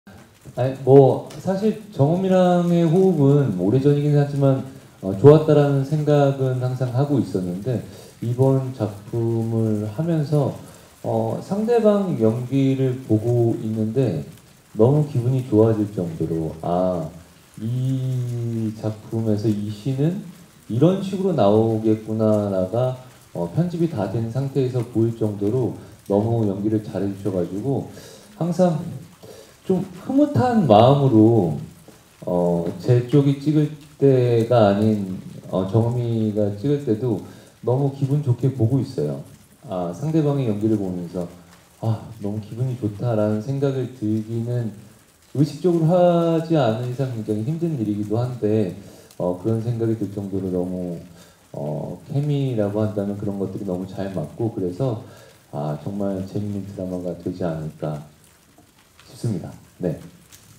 [SSTV] 남궁민, 황정음과 남매에서 연인으로… “연기 보면 기분 좋아져” (훈남정음)
[뉴스인사이드 조성욱 기자] 배우 황정음과 남궁민이 7년 만에 다시 만났다.

황정음과 남궁민이 23일 오후 서울 양천구 목동 SBS사옥에서 열린 새 수목드라마 ‘훈남정음’ 제작발표회에 참석했다.

황정음과 남궁민은 지난 2011년 드라마 ‘내 마음이 들리니’에서 남매로 출연한 이후 7년 만에 ‘훈남정음’으로 다시 만났다.

이날 남궁민은 황정음과 다시 만난 소감을 묻자 “호흡은 오래전이지만 좋았다는 생각은 항상 하고 있었다”며 “이번 작품에서 상대방의 연기를 보고 기분이 좋아질 정도로 연기를 잘했다. 항상 흐뭇한 마음으로 황정음이 찍을 때 기분 좋게 보고 있다”고 말했다.

이어 “상대방의 연기를 보면서 기분이 좋다는 생각이 들기는 의식적으로 하지 않으면 힘든데 케미가 잘 맞았다. 정말 재미있는 드라마가 되지 않을까 생각한다”고 전했다.

황정음, 남궁민, 오윤아, 최태준 등이 출연하는 ‘훈남정음’은 사랑을 거부하는 비연애주의자 훈남(남궁민 분)과 사랑을 꿈꾸지만 팍팍한 현실에 연애포기자가 된 정음(황정음 분)이 연애불능 회원들의 솔로 탈출을 도와주다가 사랑에 빠져버린 코믹 로맨스. 23일 오후 10시 첫 방송된다.